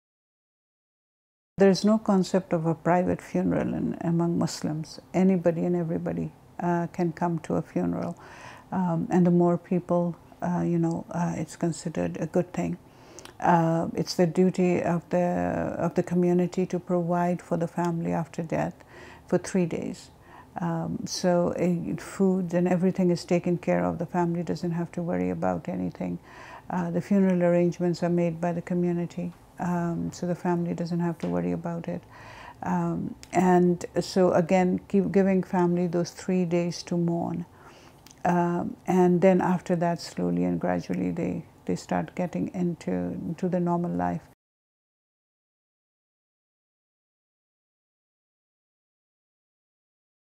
Shahina: Time to mourn
In this video by Canadian Virtual Hospice, Shahina describes how the community looks after the family for three days.